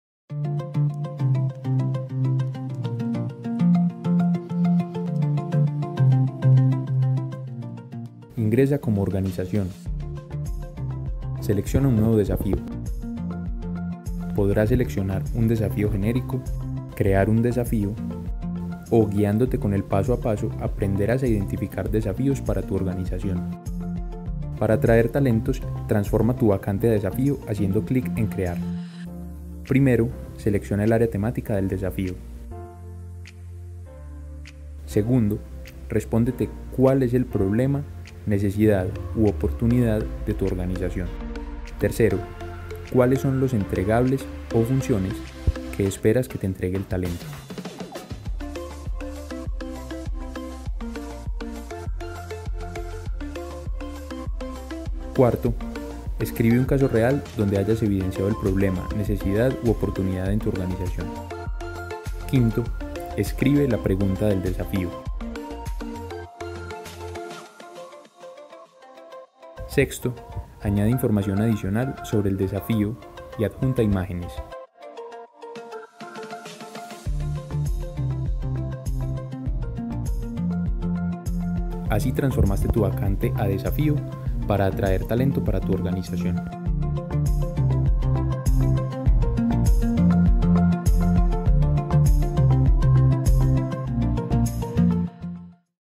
DesafíosTIColombia l Conoce cómo postular un reto a través de la Plataforma Interacpedia
Empresarios sigan el paso a paso y postulen sus retos en áreas de TI para que los estudiantes de Misión TIC los resuelvan. Conoce y selecciona el mejor Talento TI para tu empresa. Para más información sobre la convocatoria ingresa